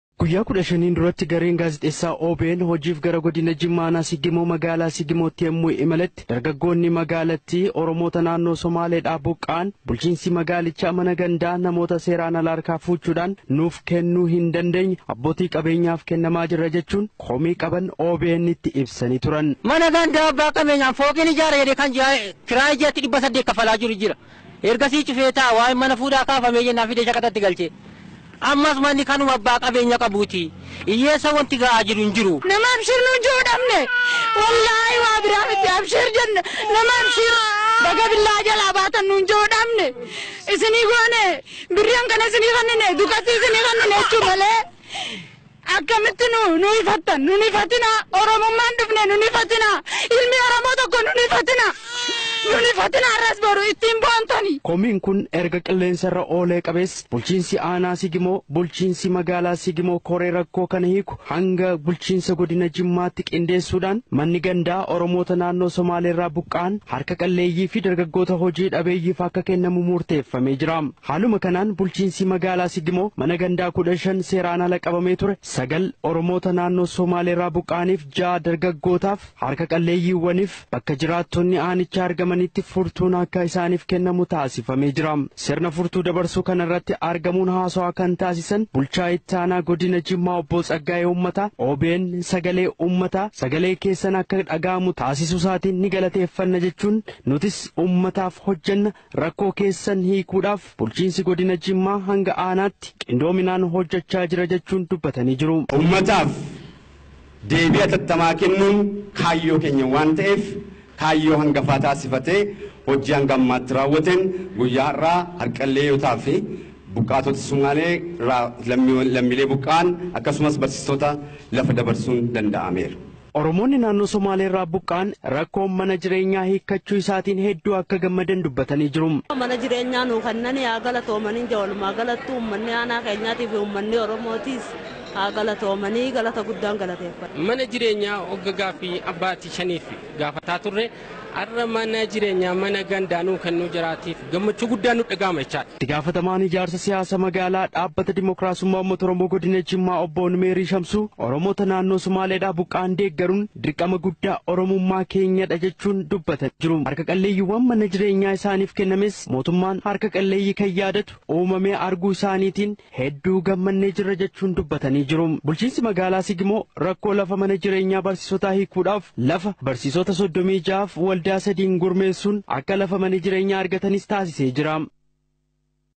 Komii Uummataa Aanaa Siigimoo Deebii argate. ===================================== [OBN 23 06 2010]
Komii Uummataa Aanaa Siigimoo Deebii argate.
[OBN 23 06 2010] manni jireenyaa jiraattoota siigimoo fi uummata qe'eerraa buqqa'aniif kenname.